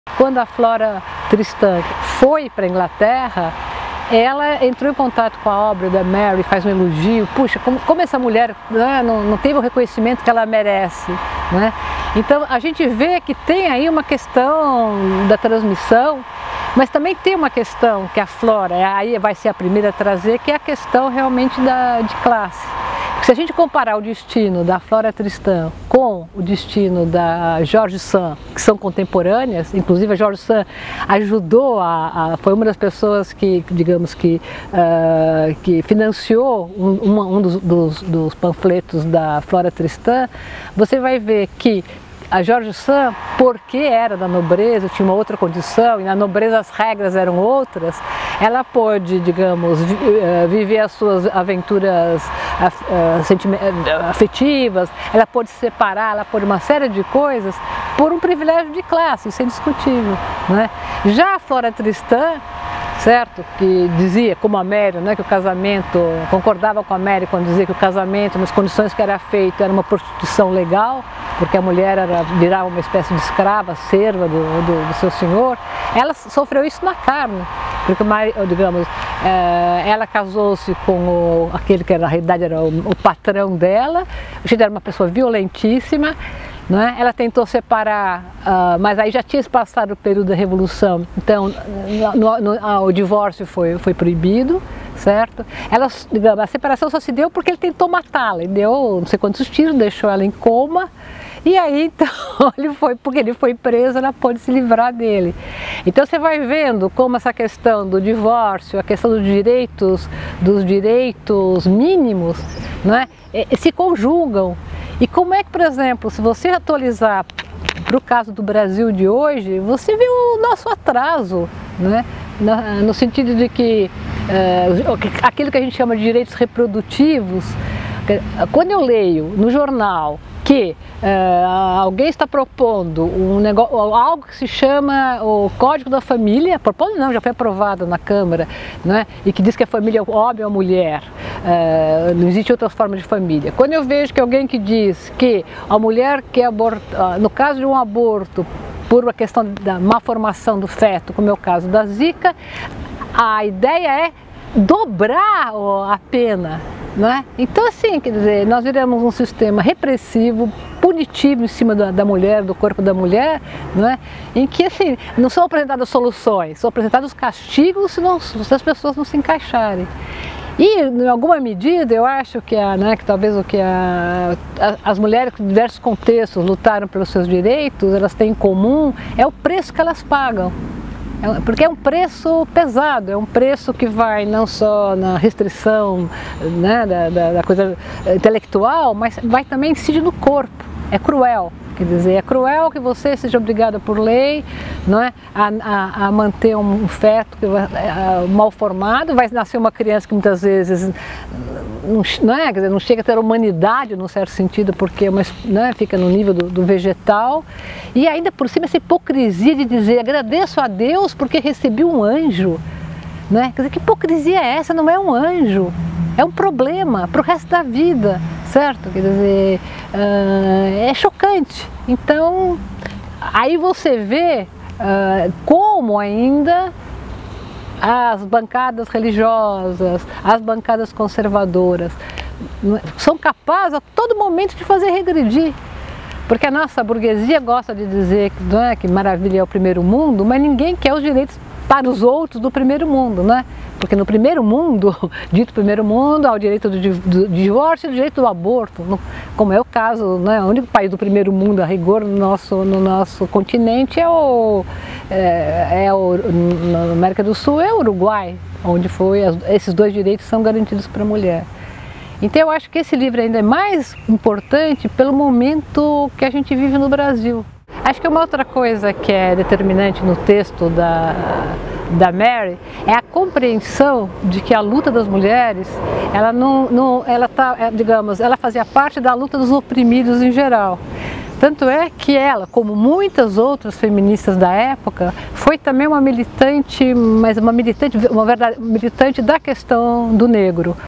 Flora Tristan, feminismo e classe (Maria Lygia Quartim de Moraes)
A socióloga Maria Lygia Quartim de Moraes discute as relações entre Flora Tristan, Olympe de Gouges, George Sand, Mary Wollstonecraft, e outras feministas dos séculos XVII e XVIII. O depoimento foi colhido no contexto do lançamento do livro "Reivindicação dos direitos da mulher", de Mary Wollstonecraft considerado um dos documentos fundadores do feminismo. A edição comentada da Boitempo deste clássico feminista vem acompanhada de um prefácio inédito escrito por Maria Lygia Quartim de Moraes.

◢ ONDE ENCONTRAR?

Para saber um pouco sobre a história e importância do livro para o feminismo hoje, não deixe de ler o texto "Mary Wollstonecraft e as origens do feminismo", de Luis Felipe Miguel, no Blog da Boitempo